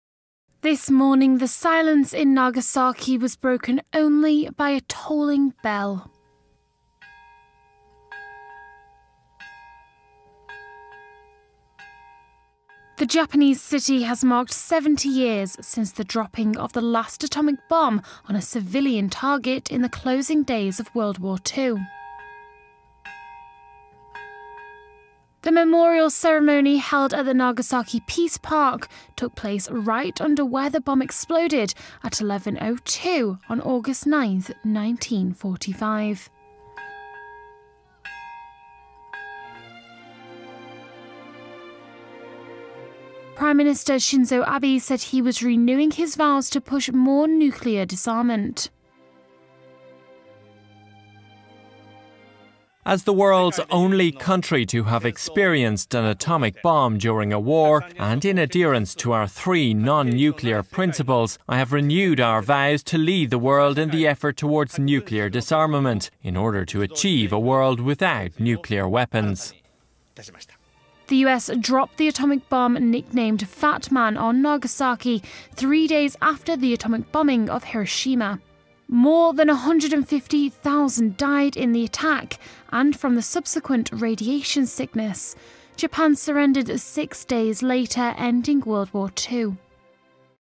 Japan marks 70th anniversary of Nagasaki atomic bomb
Commemorations are being held to mark the 70th anniversary of the atomic bomb dropped on the Japanese city of Nagasaki.
. Report by Jennifer Cordingley.